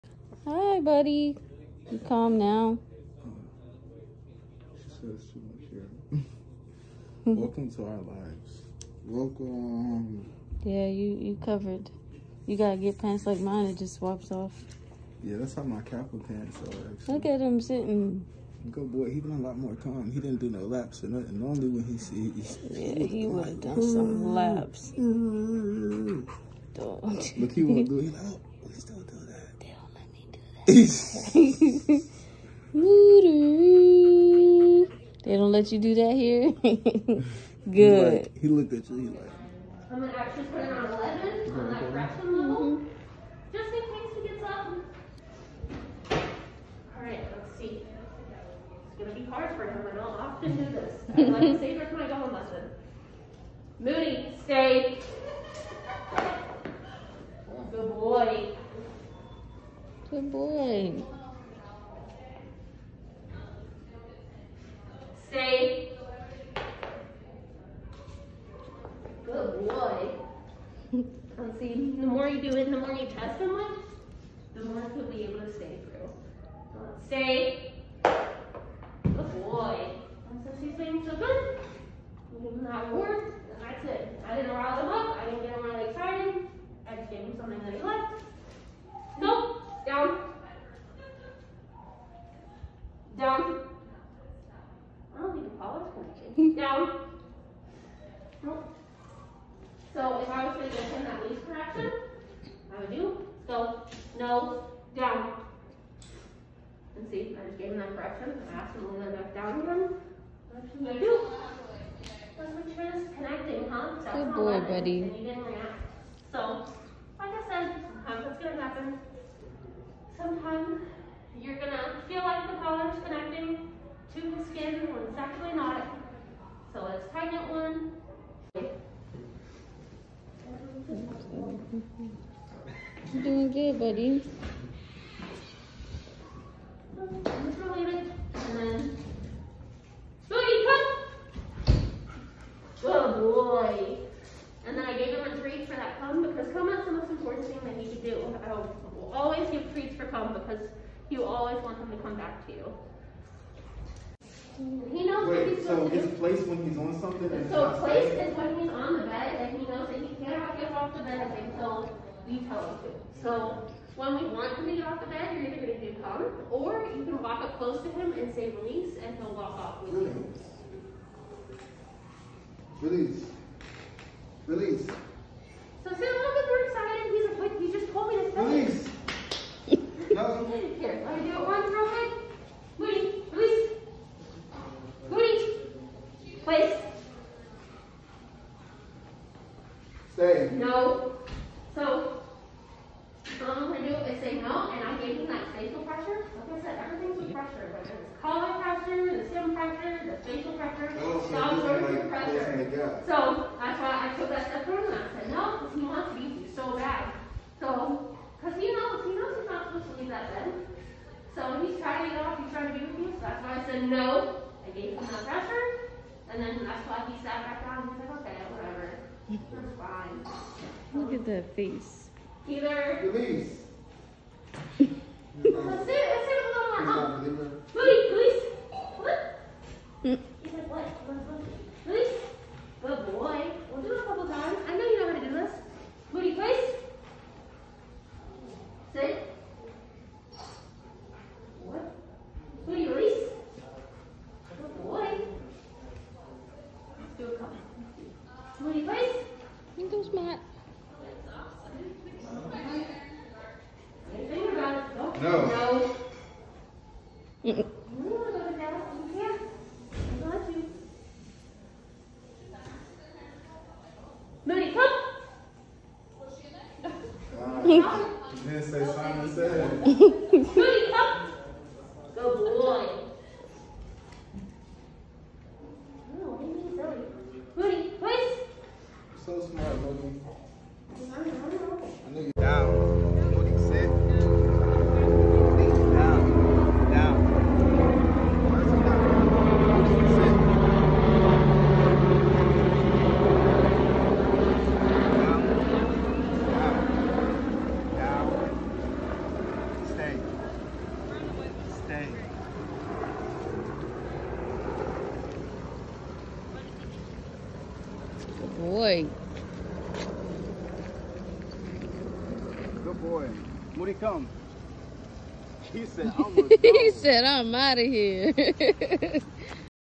Husky training
Moody shows off distraction skills.